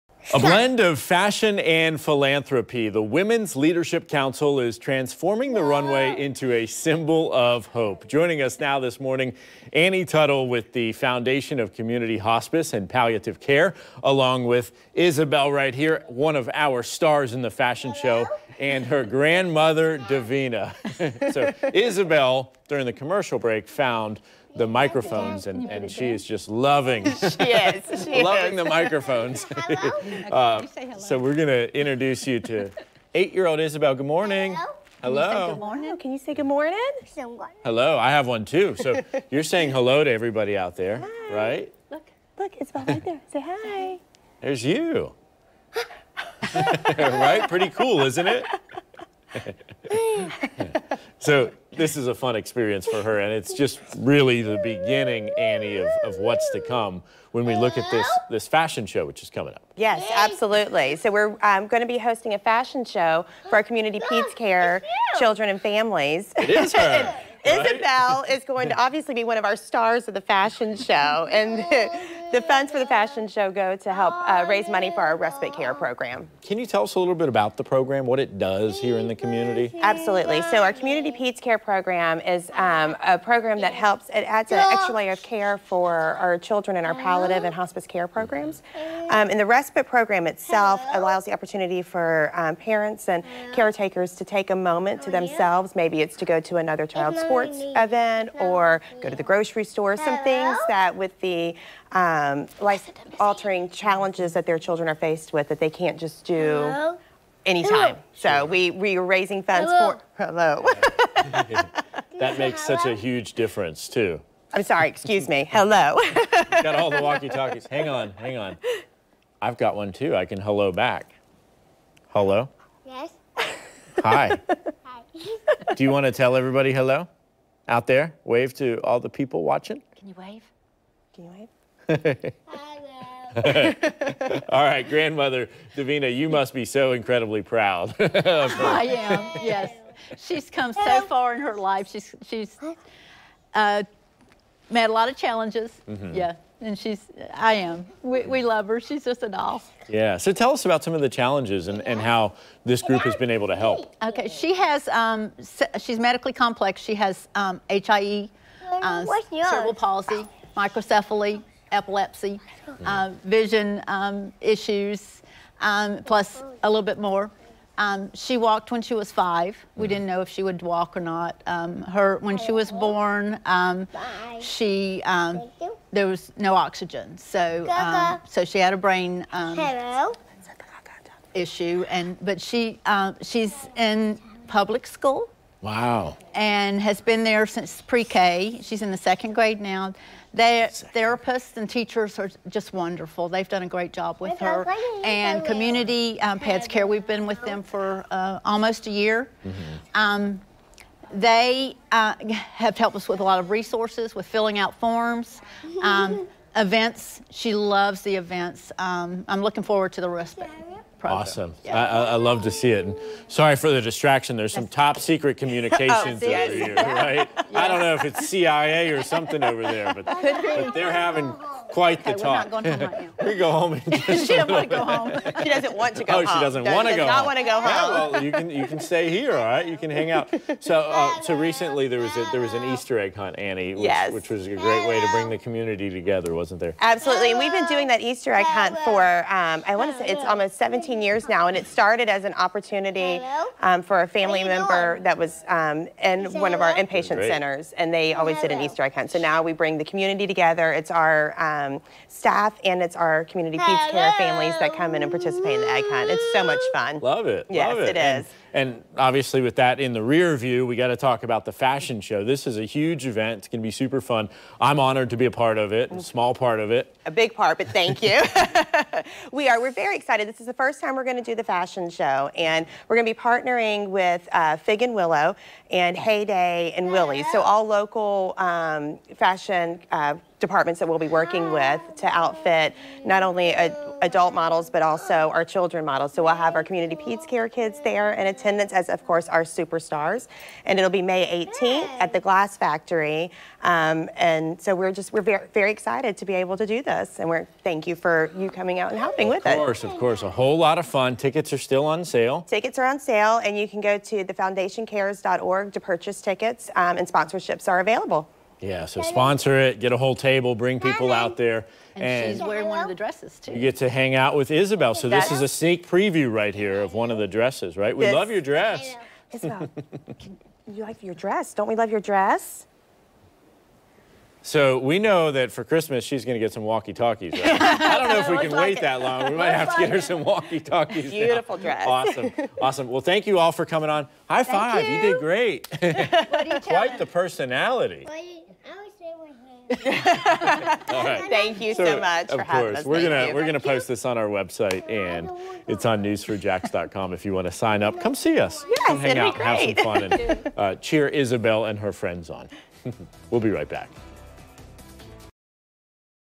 Women's Leadership Council Fashion Show
A blend of fashion and philanthropy. The Women's Leadership Council is transforming the runway into a symbol of hope.